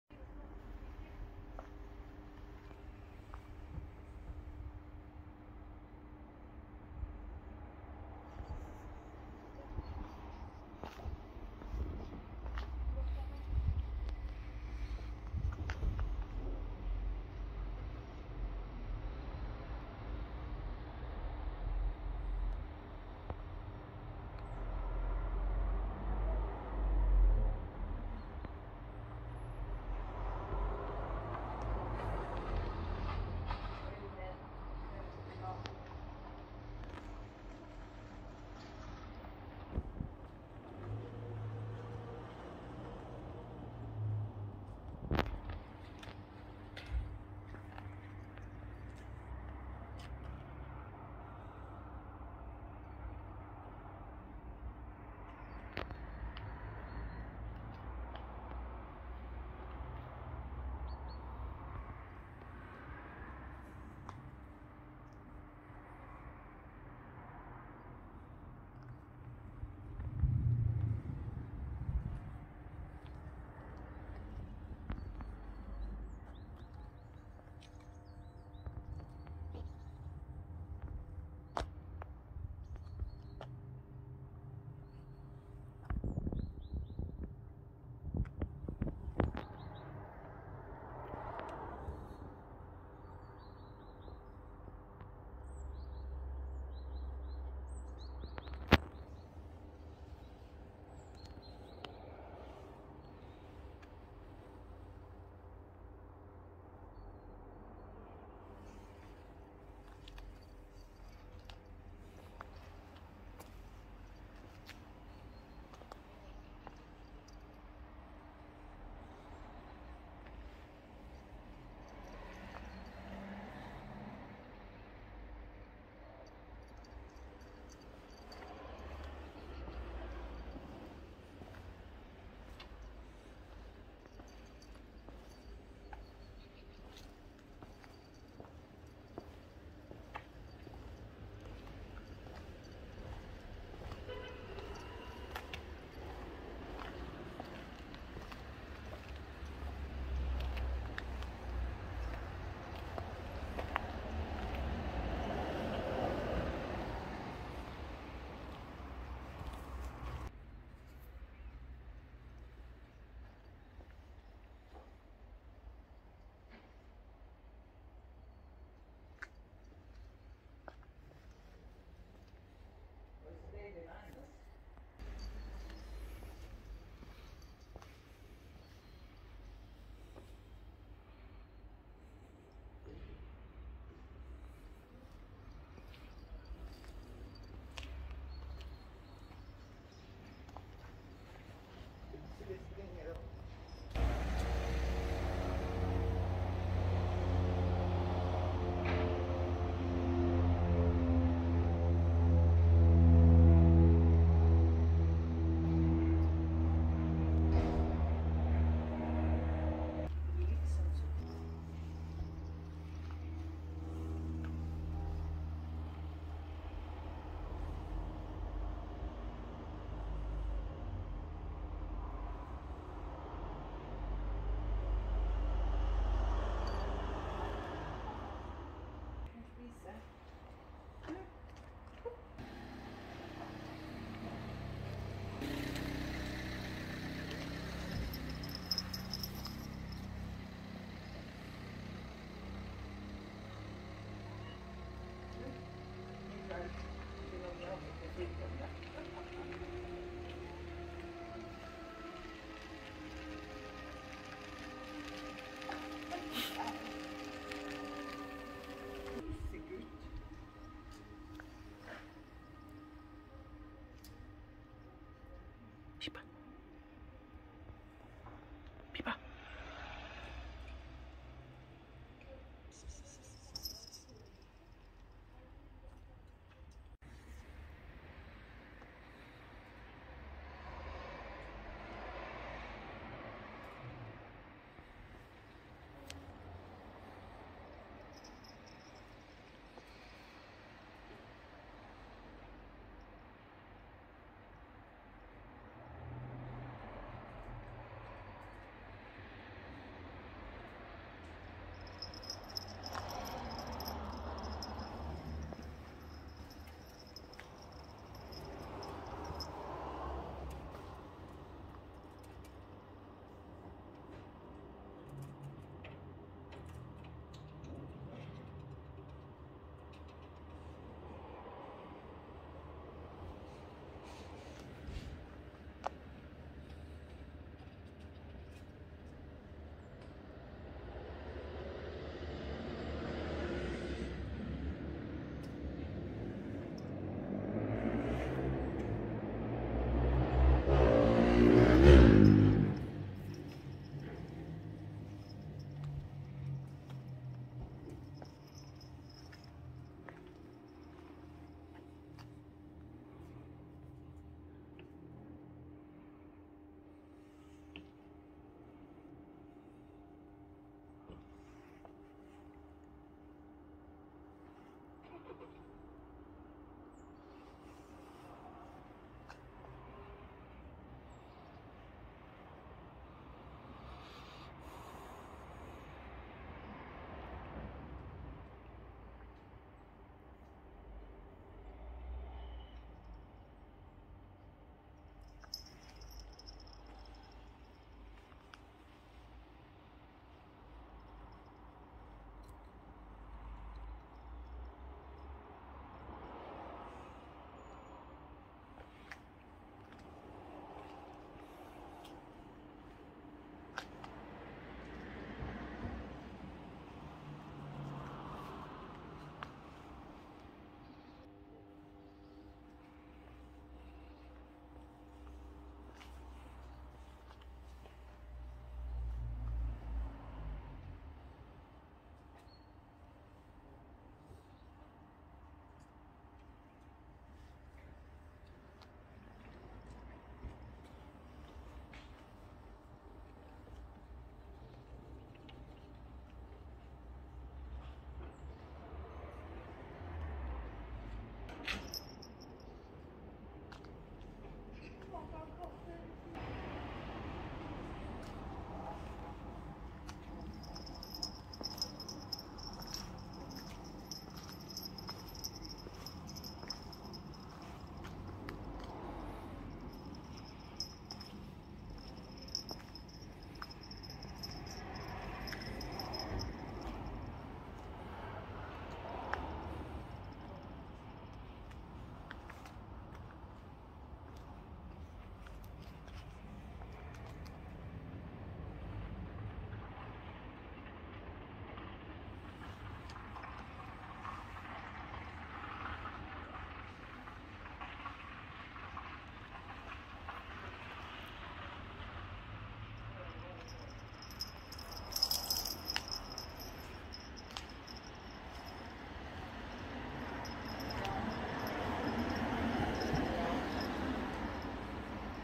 Taking our cats for a saturday walk
Fox the ginger cat ♂️🐈 and Pippa the tortie cat ♀️🦈